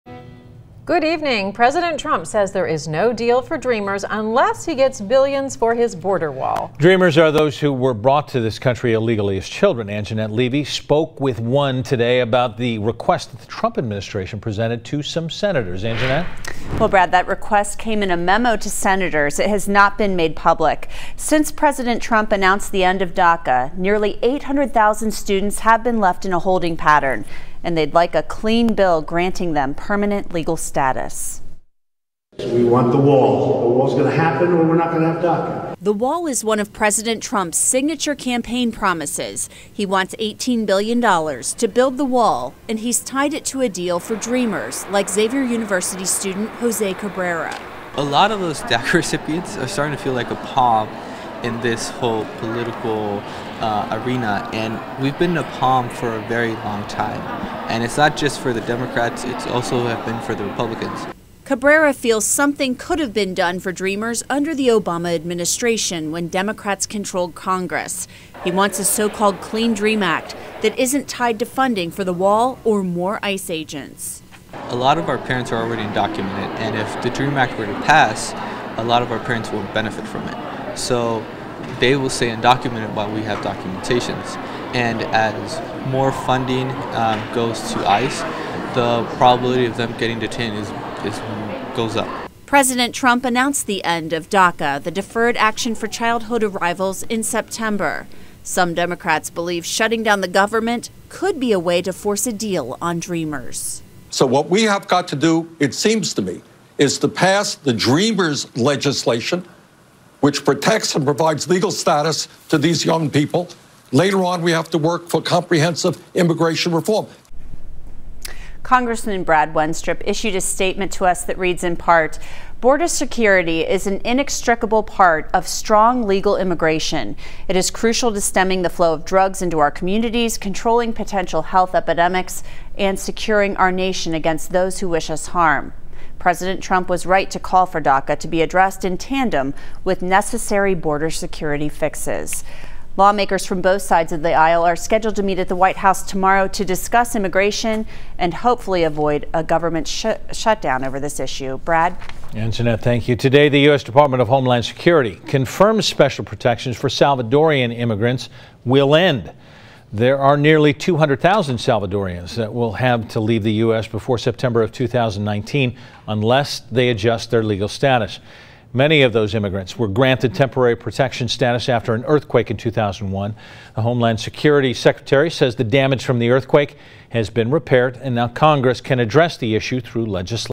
"Dreamer" says "no" to wall funding tied to DACA deal
CINCINNATI (WKRC) - President Trump reiterated his demand that a deal for "dreamers" be tied to funding for a border wall and an increase in ICE agents. The Dept. of Homeland Security sent a memo to some senators late last week that stated $18 billion was needed to build portions of the wall along the border with Mexico.

"We want the wall. The wall's going to happen or we're not going to have DACA," President Trump said over the weekend at Camp David.

But Jose Cabrera, a student at Xavier University who is also a DACA recipient, says dreamers want a "clean" Dream Act that does not including funding for the wall.

"A lot of the DACA recipients are starting to feel like a pawn in this whole political arena and we've been a pawn for a very long time and it's not just for the Democrats it's also for the Republicans," Cabrera said Monday.

Cabrera said Democrats could have done something for DACA recipients under the Obama Administration when Democrats controlled Congress. Cabrera said he believes Democrats have used dreamers to get support from Latino voters. He said dreamers don't support increases in funding for ICE agents because it could impact their parents.

"They will stay undocumented while we have documentation and as more funding goes to ICE the probability of them getting detained goes up," Cabrera said.

President Trump announced the end of DACA - the Deferred Action for Childhood Arrivals - in September. DACA was created through an executive order by President Obama. Many legal experts believed the order was unconstitutional. Some Democrats believe forcing a government shutdown could be a way to strike a deal with dreamers.

"So what we have got to do, it seems to me, is to pass the Dreamer's legislation, which protects and provides legal status to these young people. Later on, we have to work for comprehensive immigration reform," said Vermont Sen. Bernie Sanders (I) in an interview Sunday.

Congressman Brad Wenstrup (R) represents Ohio's 2nd congressional district. He issued the following statement to Local 12 News Monday:

Any potential DACA deal must address how we intend to prevent this situation from happening to another generation of children. My grandfather came to the US through Ellis Island, became a citizen, and was proud to be a part of this great and diverse nation. Two of my interpreters from Iraq are now working physicians and US citizens. We need immigration, and we need good American citizens, those two needs go hand-in-hand. DACA, President Obama’s executive order, did not provide a long-term solution for children brought into this country by their parents. Even the act’s strongest proponents admit that it is constitutionally weak and was likely to get defeated in court. Border security is an inextricable part of strong, legal immigration. It is crucial to stemming the flow of drugs into our communities, controlling potential health epidemics, and securing our nation against those who wish us harm. President Trump was right to call for DACA to be addressed in tandem with necessary border security fixes.
Democratic and Republican lawmakers are scheduled to meet at the White House on Tuesday to discuss a possible deal on DACA.